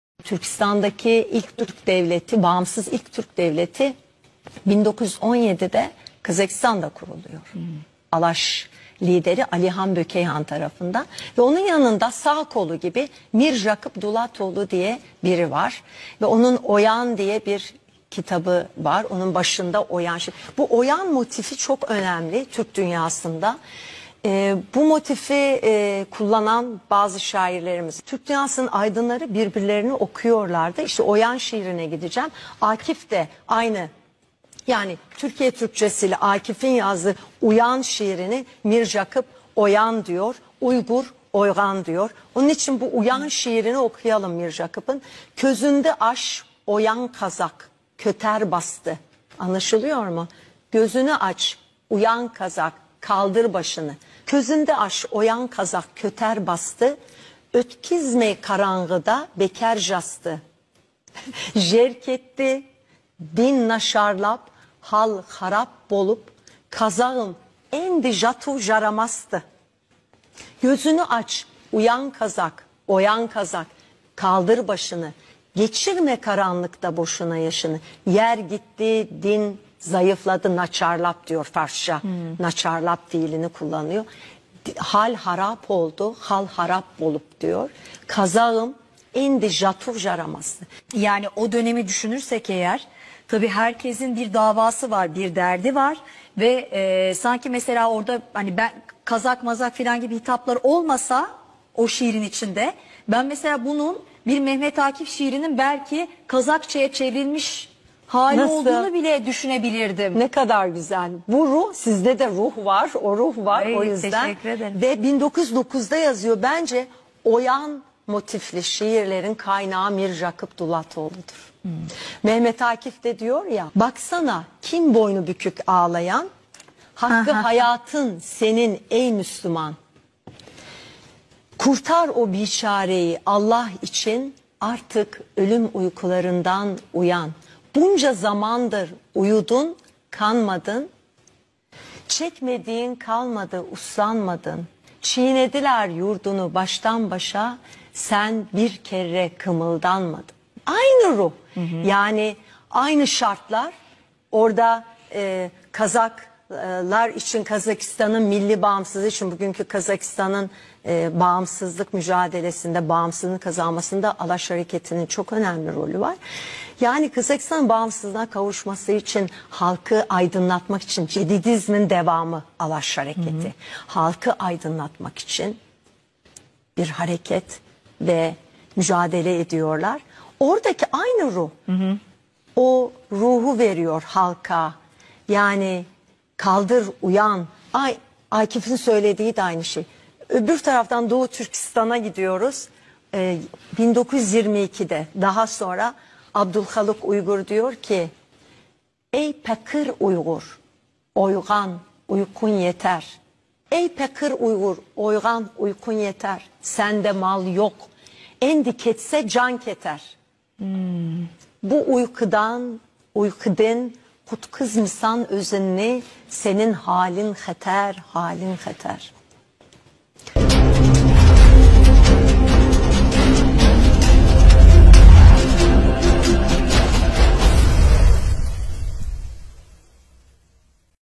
Prof. Dr. Hülya Kasapoğlundan Miryakup Devletoğlunun (Mirjakıp Dulatulı) "Uyan Kazak" şiiri 🇰🇿🇹🇷
Түрік профессоры Міржақып Дулатұлының “Оян, қазақ” өлеңі жайында

токтар аубакиров
dombra kazak kızı
dombıra küyü
dombra küyü
dombıra
türkiye
kazakça